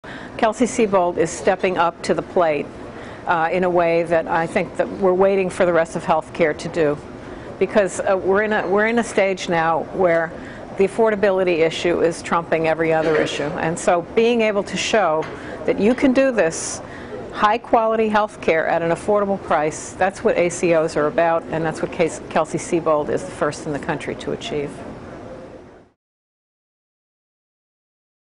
NCQA President Margaret O'Kane Discusses Kelsey-Seybold's ACO Accreditation
NCQA President Margaret O'Kane discusses how Kelsey-Seybold Clinic is the first accountable care organization in the nation to receive NCQA ACO accreditation.